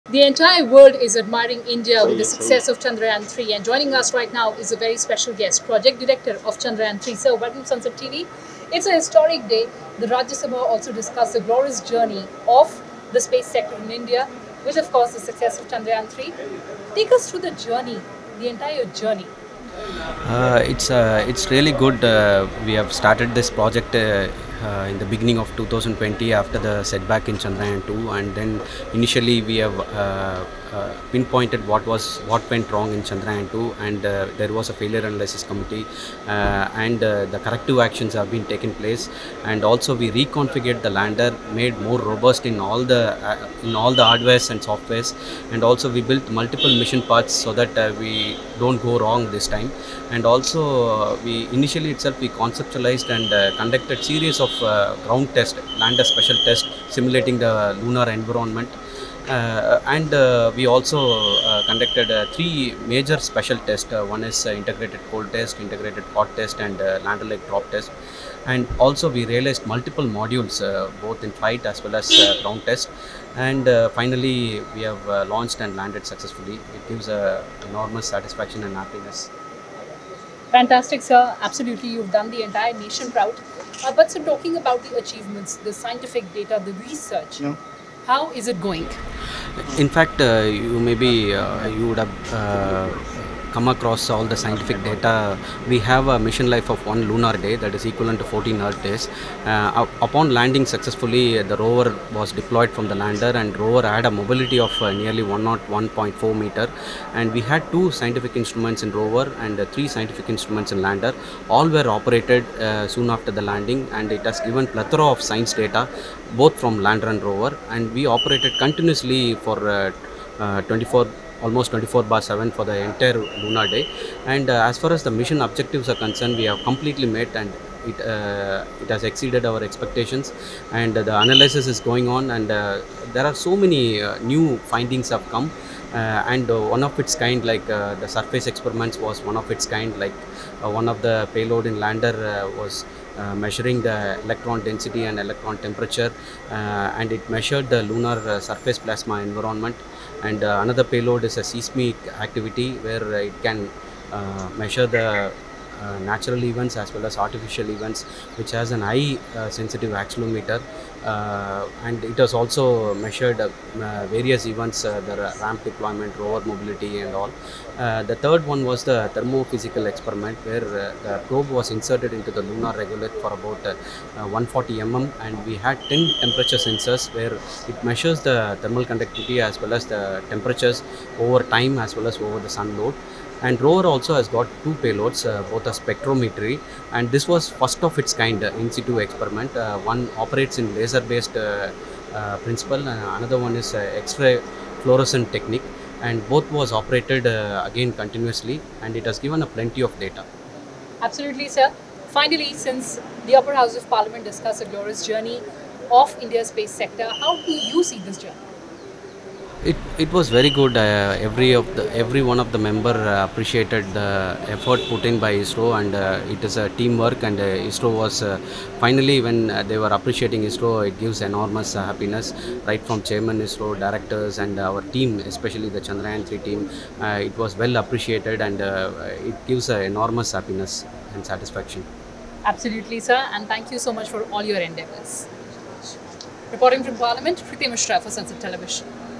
P Veeramuthuvel, Project Director of Chandrayaan-3 Mission speaks exclusively to Sansad TV
The Upper House took up discussion on India's glorious space journey marked by the successful soft landing of Chandrayaan-3. P Veeramuthuvel, Project Director of Chandrayaan-3 Mission speaks exclusively to Sansad TV.

Anchor- Kriti